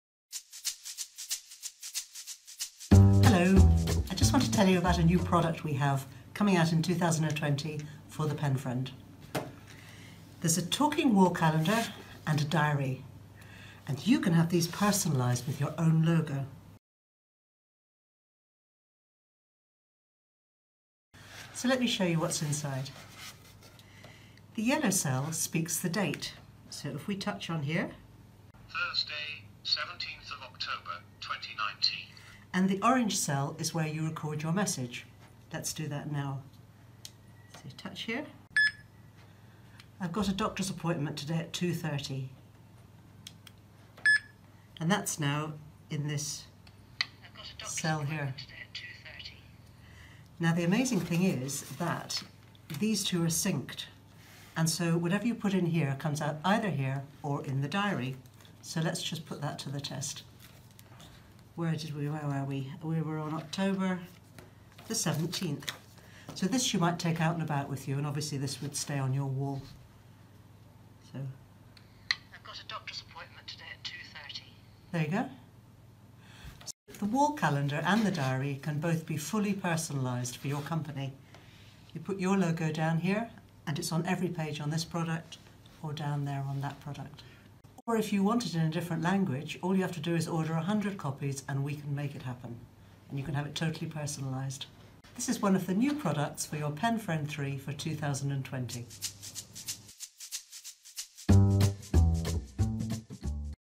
Personalised Talking Wall Calendar and Desk Diary with PENfriend
Another use for the PENfriend - the labelling device for visually impaired people. Make a recording on any day in the calendar and playback on the same day in the diary; and vice versa.
TAP YELLOW box on any day and listen to audio narration of day, date, month and year. TAP ORANGE box to listen to your recording.